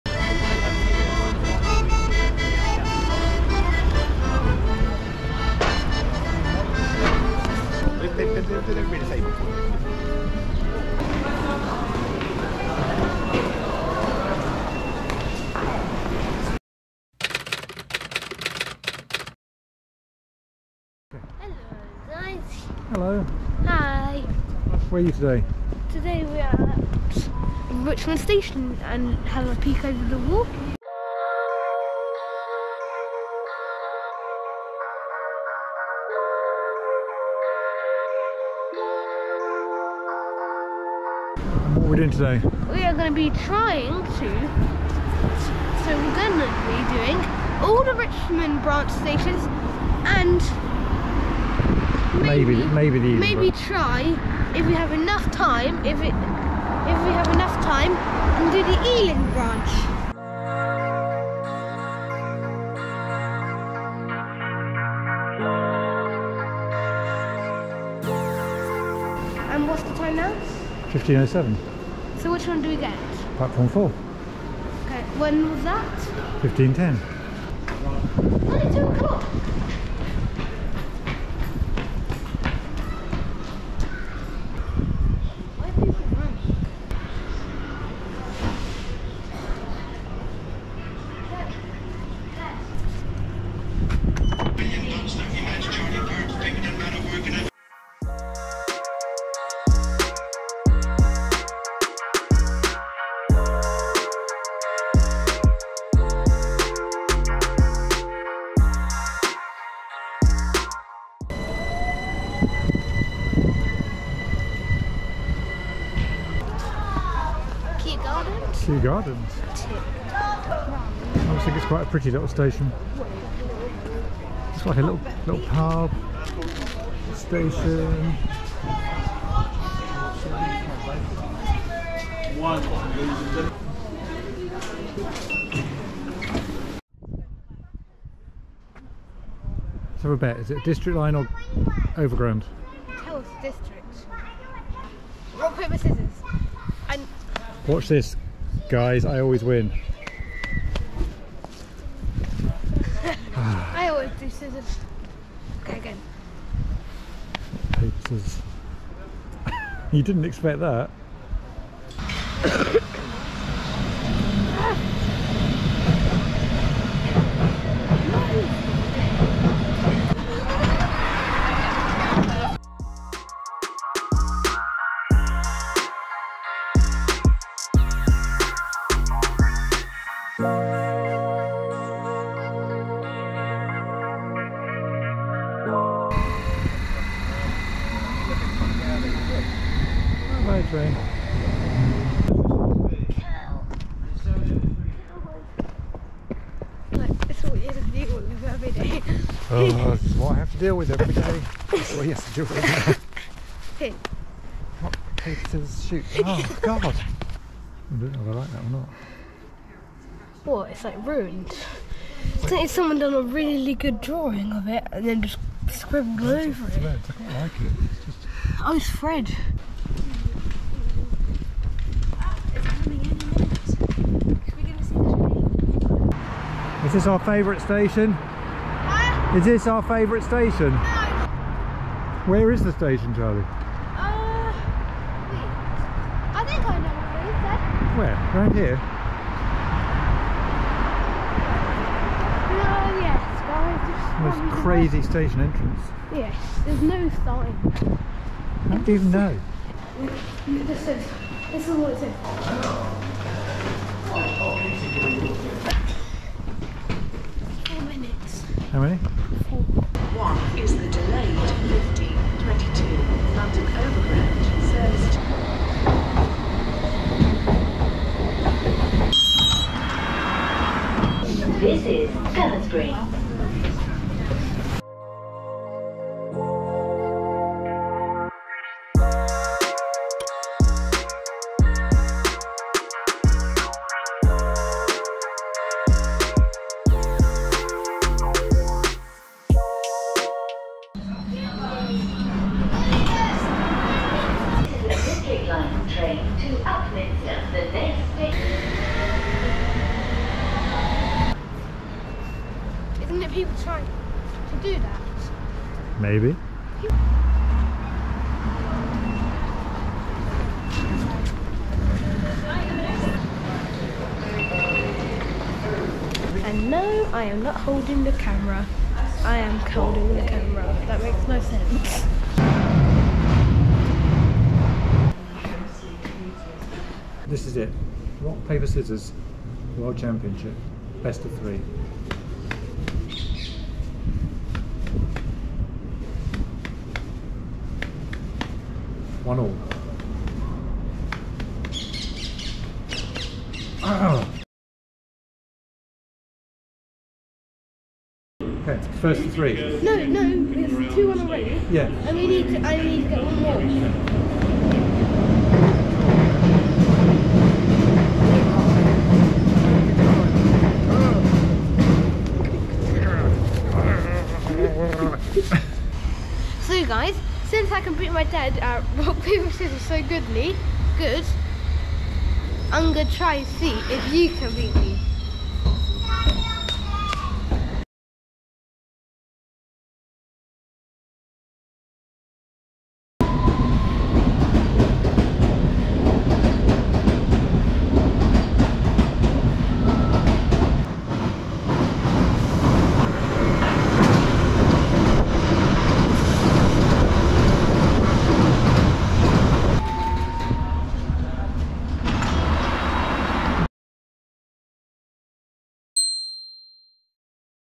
All the stations to Richmond - Exploring the District Line
Continuing with our mission to explore the District Line we ticked off the (very short) branch line to Richmond. In fact we started in Richmond and went to Turnham Green, via Kew Gardens and Gunnersbury. Oh, and Charlie is in the middle of a Rock, Paper, Scissors moment so there's a few challenges along the way.

📖 CHAPTERS
00:00 Introduction & Richmond Station
01:51 Kew Gardens
03:05 Gunnersbury
04:51 Turnham Green
05:26 Rock Paper Scissors Challenge
06:23 Outro

🌎 Richmond to Turnham Green stations

🎥 Filmed on a GoPro Hero10